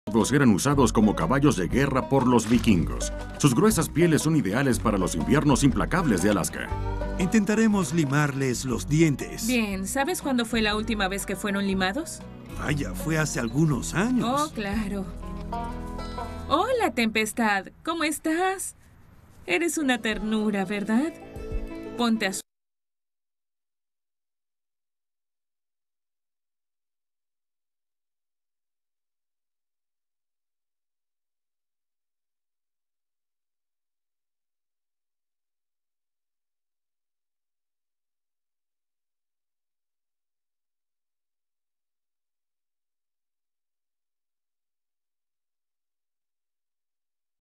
Cierre de Nat Geo Wild Latinoamérica • 01.04.22
Prácticamente fue el único cierre que Claro dejó de origen porque los demás los cambiaron minutos antes, y de paso, fue el único canal que quedó sin reemplazo...